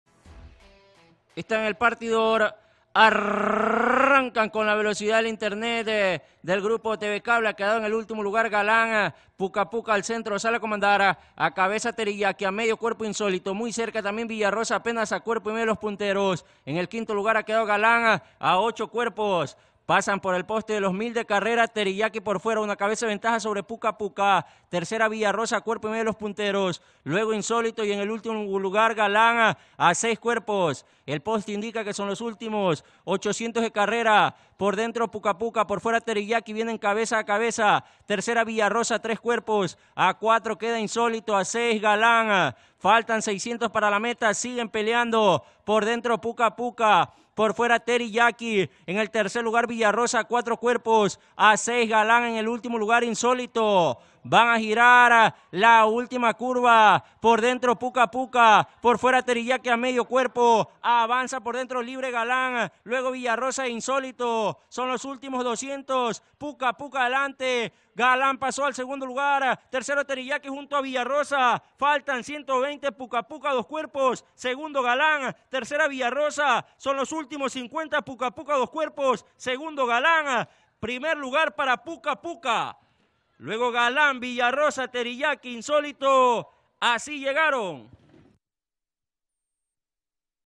2021-100 Puca Puca (Ecu) abr. 4, 2021 - Hipód. Miguel Salem Dibo - Revista La Fija
PUCA PUCA (Ecu-2018) hembra castaña por Golden Itiz y Forastera (High Cotton). Tercera victoria en 4 salidas. Jinete: Luis Hurtado (Per); Preparador: Armando Roncancio (Col); Stud: Chipipe. Haras: Don Miguel. Distancia 1.300 metros. Tiempo: 1’20”3 (pista liviana).
1º) Puca Puca (Ecu), 2º) Galán (Ecu), 3º) Villa Rosa (Ecu). Todos corrieron.